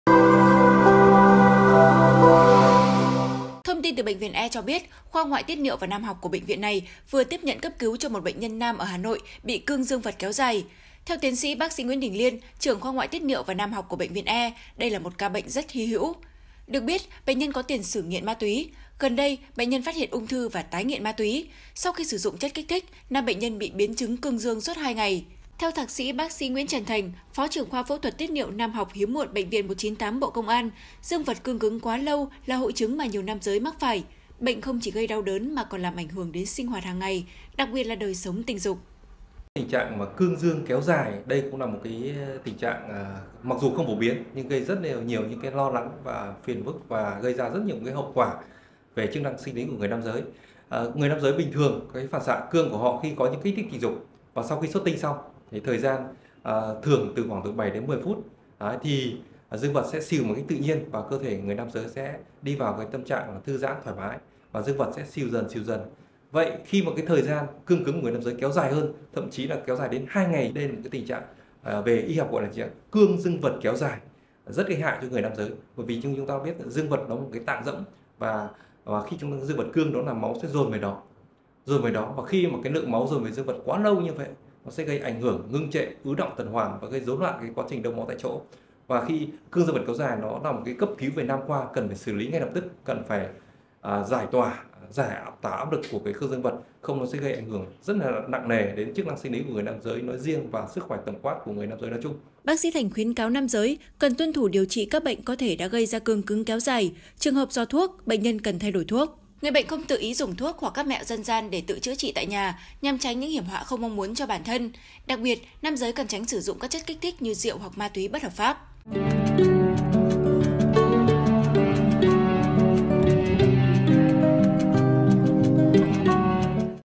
Cấp Cứu Người Đàn Ông Bị Cương Cứng "Cậu Nhỏ" Suốt 2 Ngày Do Dùng Chất Kích Thích | SKĐS
SKĐS | Sau khi sử dụng chất kích thích, nam bệnh nhân bị biến chứng cương dương suốt 2 ngày. Bác sĩ cho biết đây là một ca bệnh hết sức hy hữu.

Mời quý vị và các bạn xem thêm: Tự Ý Dừng Thuốc Điều Trị, Người Đàn Ông Bùng Phát Bệnh Nấm Phổi